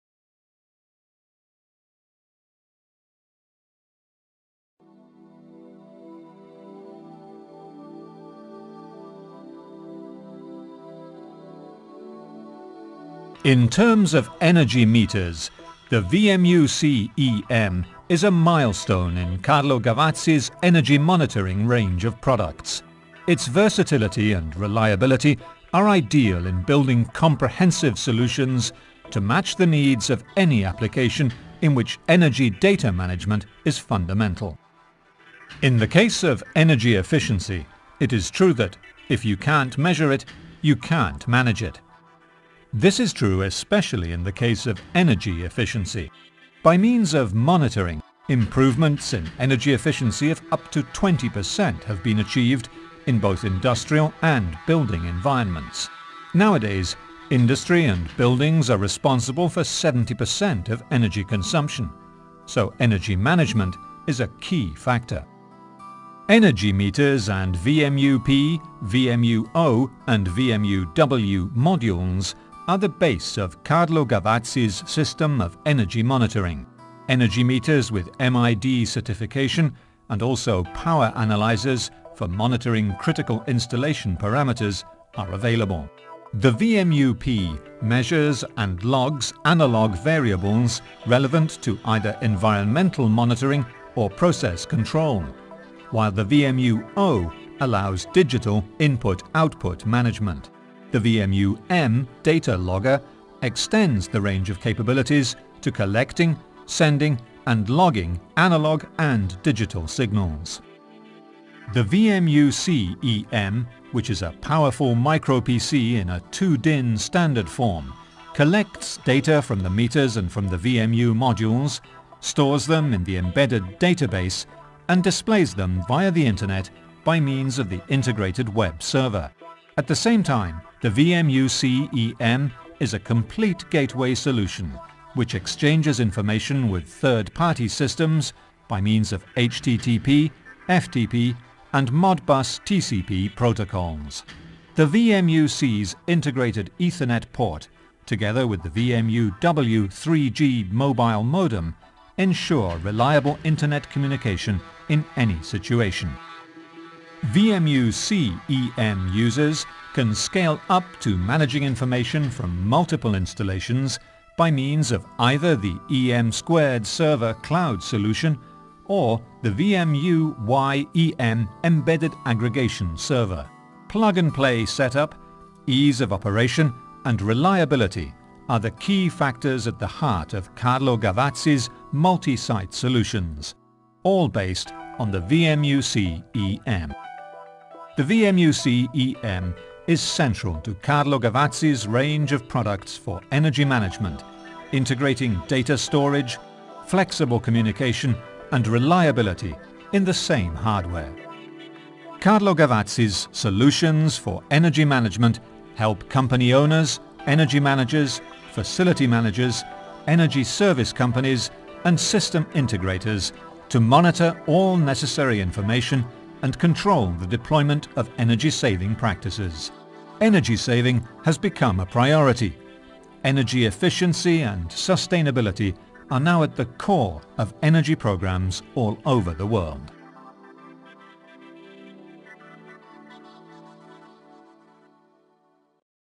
VMUC EM From CARLO GAVAZZI
to get more information on Carlo Gavazzi products. Call for more information Manufacturers Automation: Toll Free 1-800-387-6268

The VMUC-EM collects electrical, digital and analog data from multiple energy meters and makes the data available via Modbus TCP, HTTP (web server) and FTP.